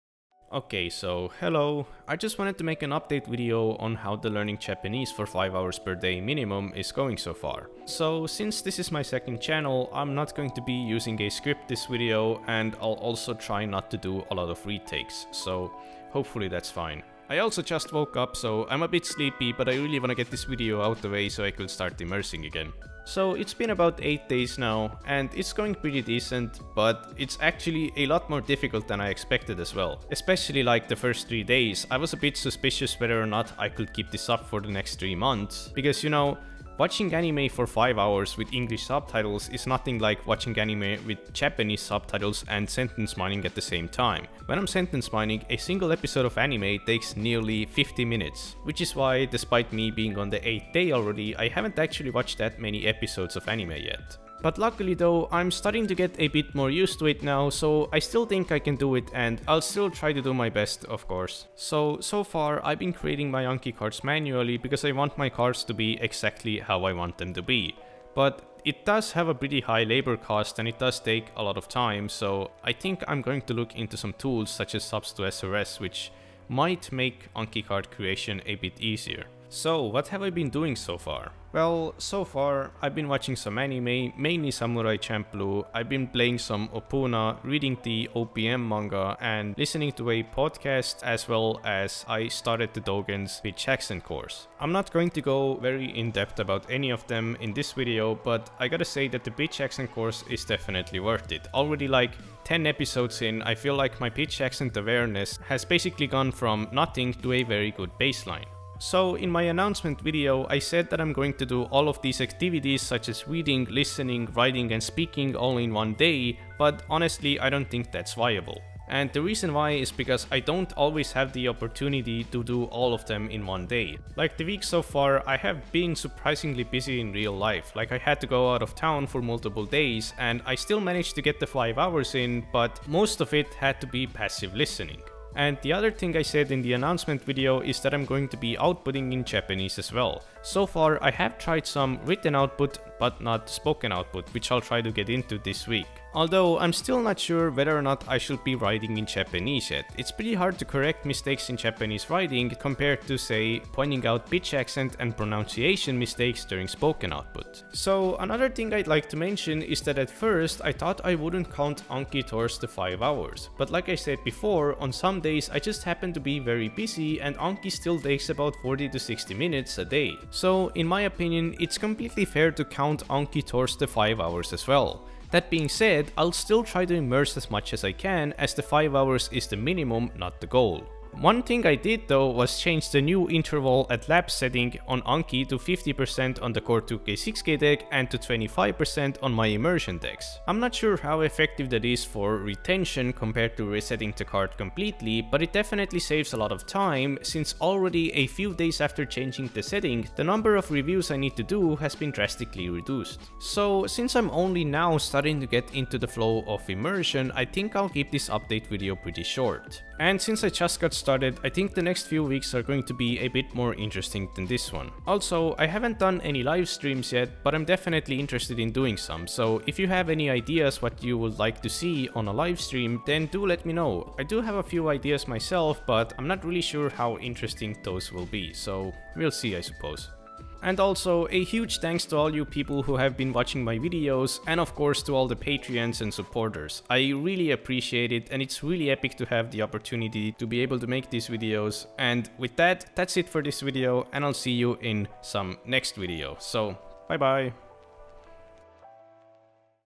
First Week of Learning Japanese for 5 Hours a Day Update
So the first 8 days of learning has passed, and so far I have successfully immersed/learned for 5 hours (minimum) a day. Unfortunately I was really busy this week and couldn't get the ball rolling as well as I hoped, but no worries as its only the beginning.